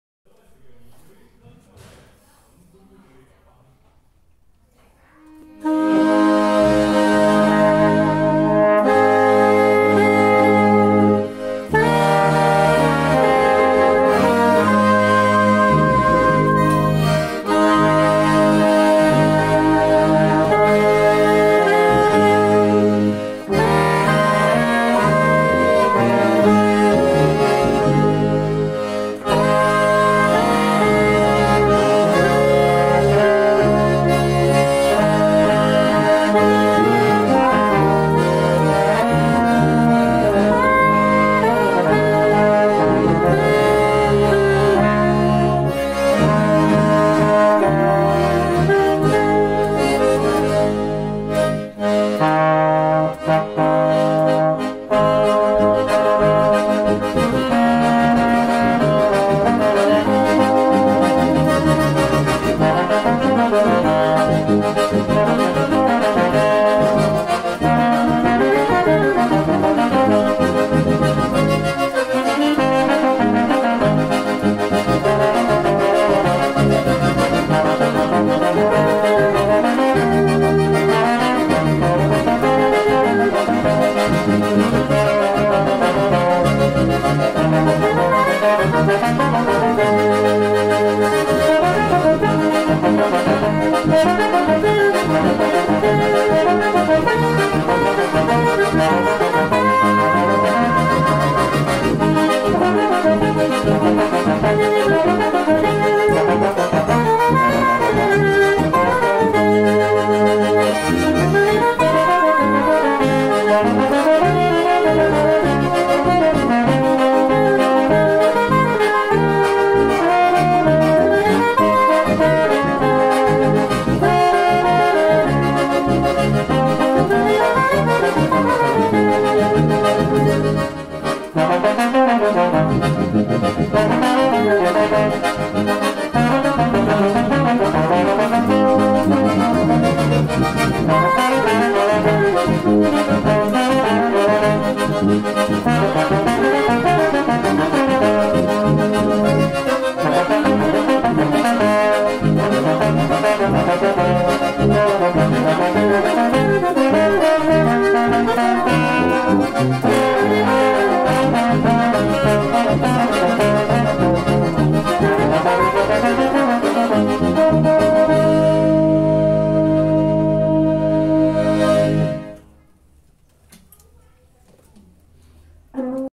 Tocata São José dos Pinhais(1) 🎶🎵 Visita do Matheus tubista do MT Grosso
Matheus - Tuba
Fábio- Trombone
Arildo - Acordeon
Samuel - Saxofone